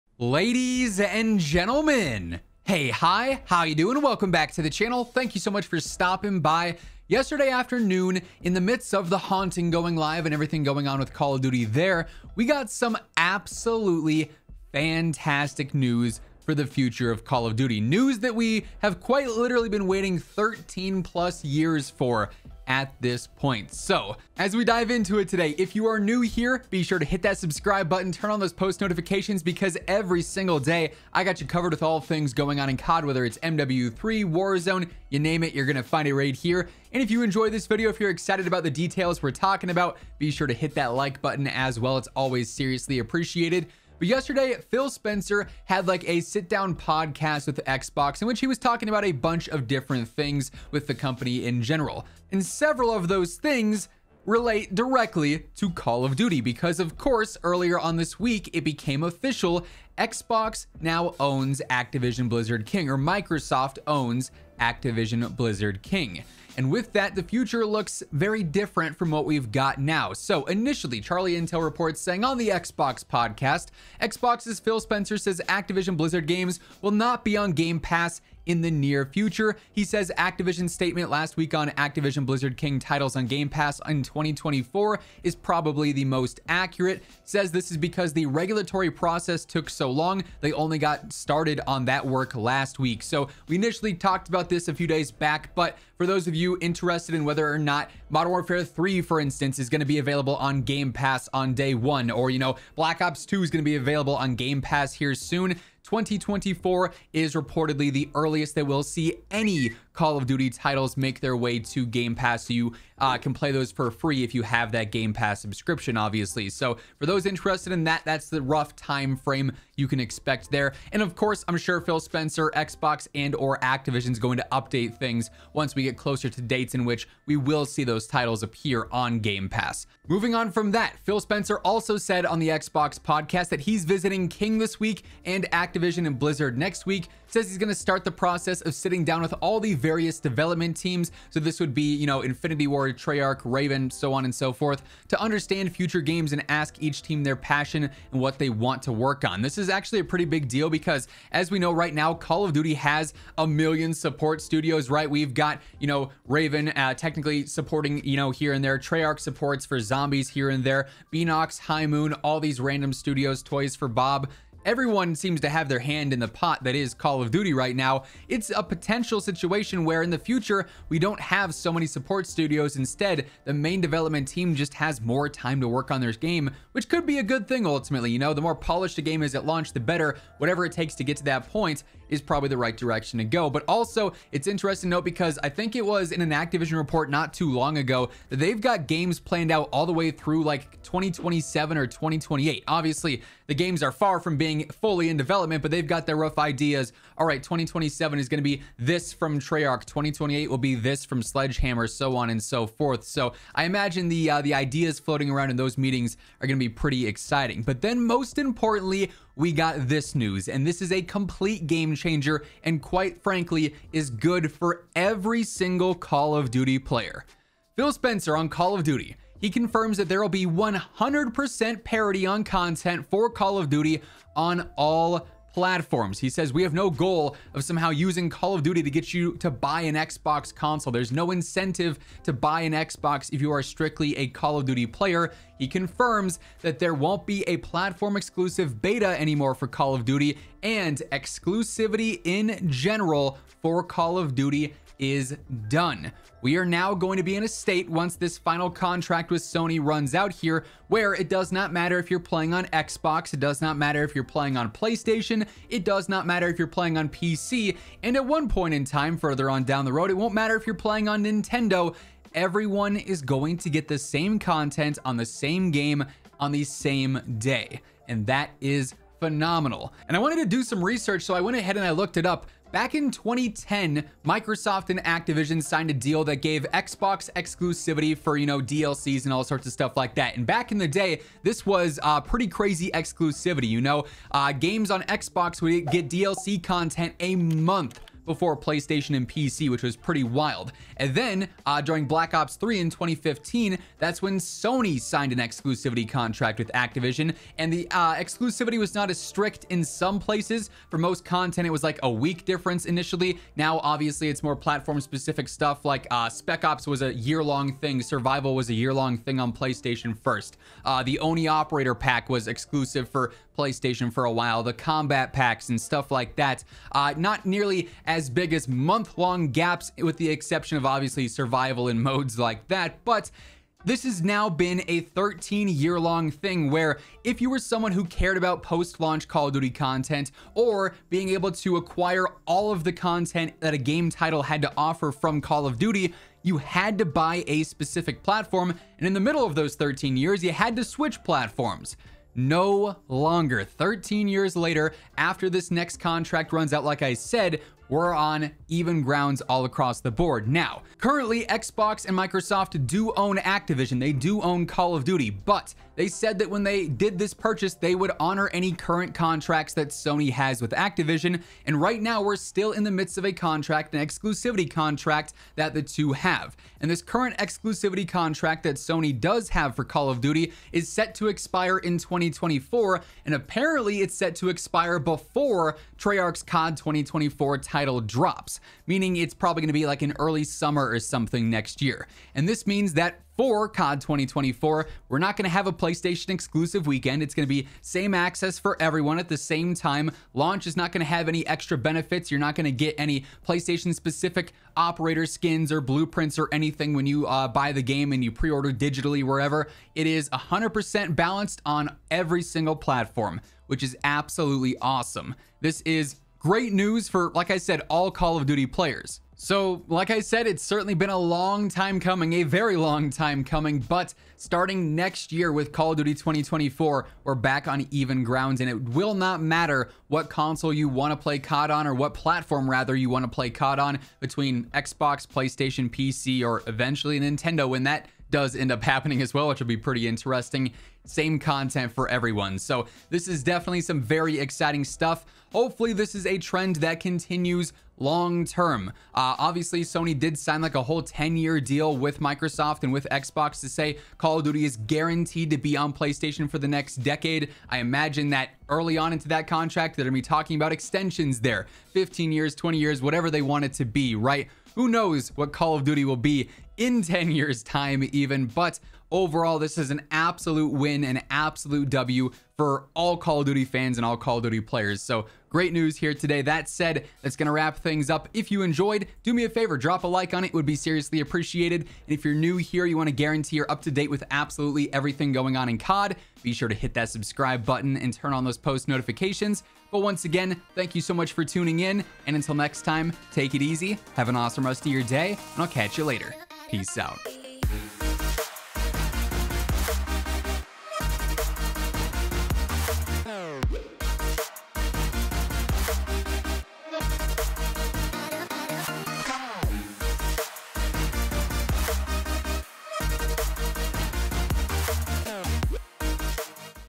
Call Of Duty EXCLUSIVITY Is COMPLETELY ENDING!
COD Is Finally Ending EXCLUSIVITY!

0:00 – COD Just Got Some GREAT NEWS
0:45 – Xbox Reveals New Details About Call of Duty’s Future
3:27 – Call Of Duty Exclusivity Is DONE!

In this video, we're breaking down the GREAT NEWS that Call of Duty just Revealed regarding Exclusivity! With Microsoft taking over Call of Duty, it is now official that after Modern Warfare 3 Launches and we approach COD 2024, COD Exclusivity is ending!